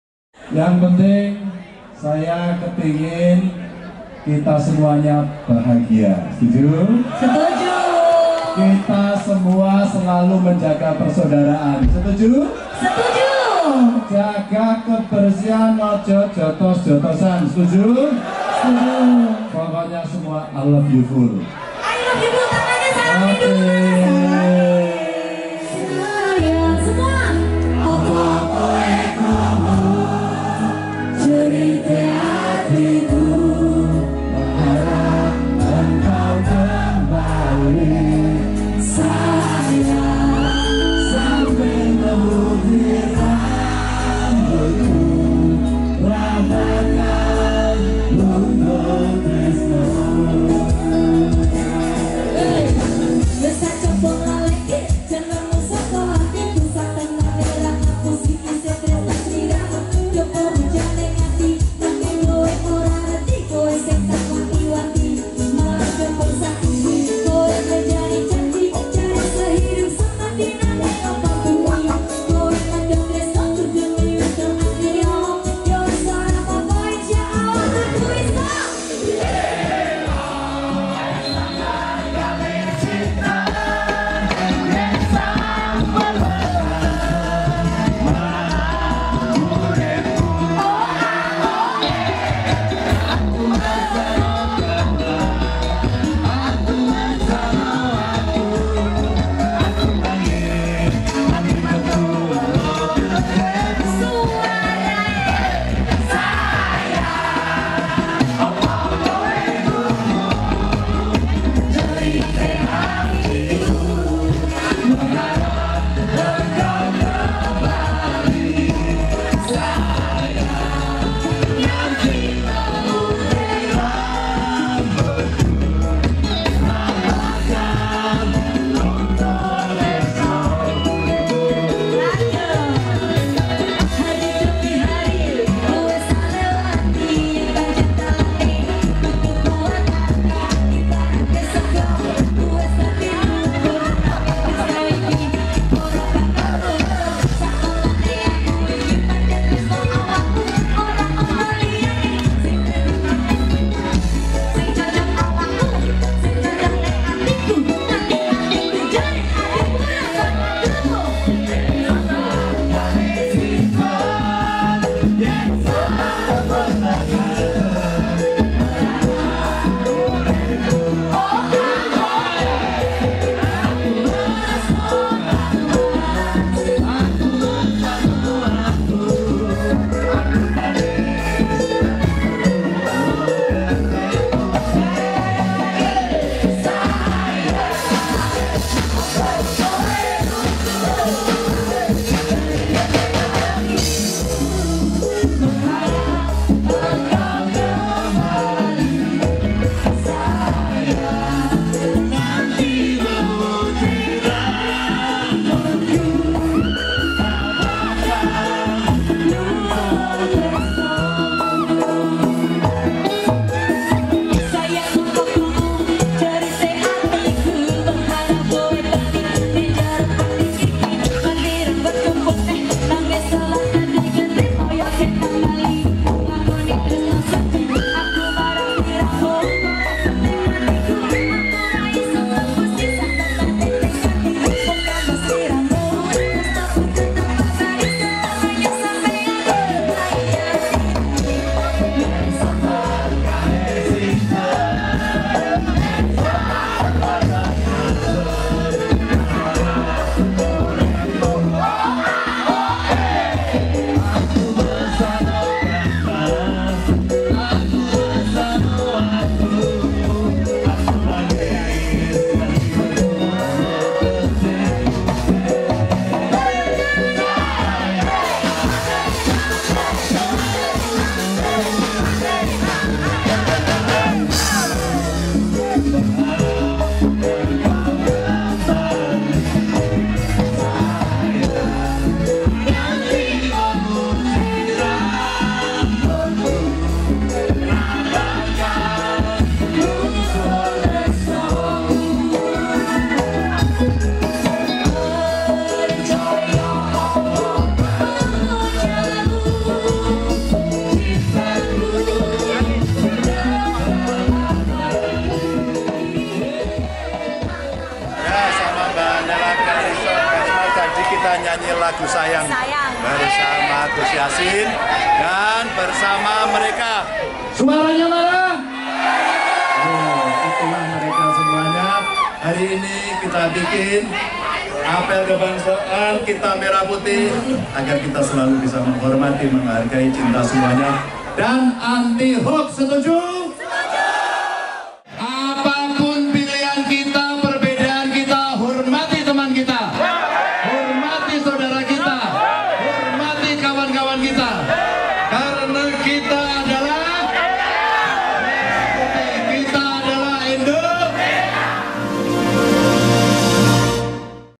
MERINDING LIHAT GANJAR NYANYI DENGAN RIBUAN PENDUKUNGNYA
H. Ganjar Pranowo, S.H., M.I.P. adalah seorang politisi yang saat ini menjabat sebagai Gubernur Jawa Tengah selama dua periode sejak 23 Agustus 2013. Sebelumnya, ia merupakan anggota Dewan Perwakilan Rakyat dari Fraksi PDI Perjuangan periode 2004–2009 dan 2009–2013.